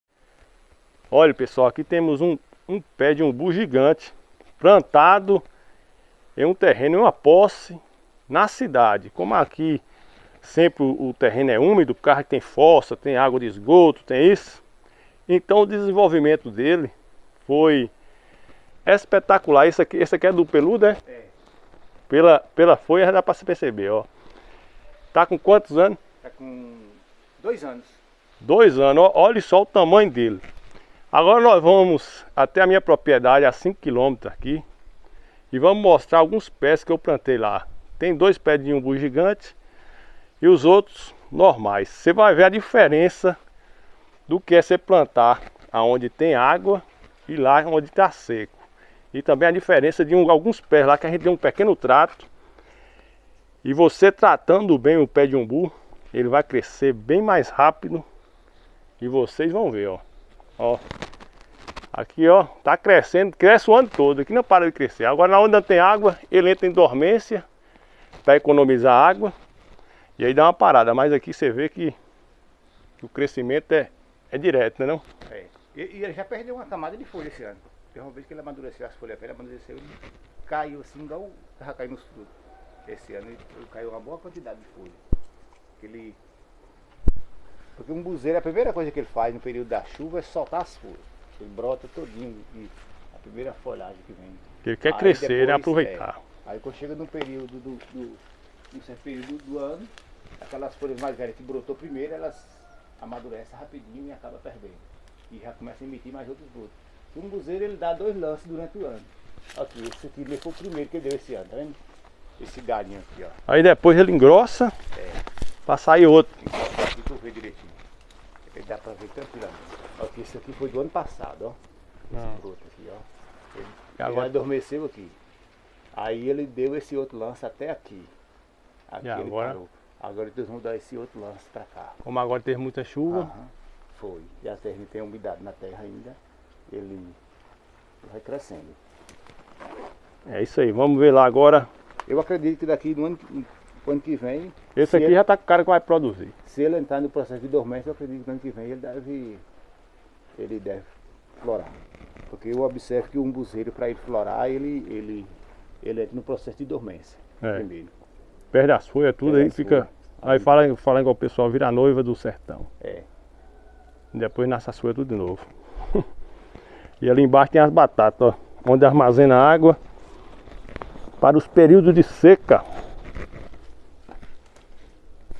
Umbuzeiro Plantado Na Cidade
Cresceu bastante o Umbuzeiro plantado na cidade.
Quer comprar mudas de Umbu?
Aqui em Paramirim Bahia tem um viveiro onde você pode adquirir mudas de Umbu Gigante para plantar aí na sua propriedade.  São cinco variedades. No vídeo o proprietário do viveiro (Gilberto Silva Almeida) mostra as mudas e explica tudo o que você precisa saber sobre as mudas e o plantio. Gilberto tem experiência na área, tem uma propriedade piloto com duzentas mudas plantadas na comunidade de Noruega em Paramirim, e trabalha na Escola Agrícola na cidade de Boquira.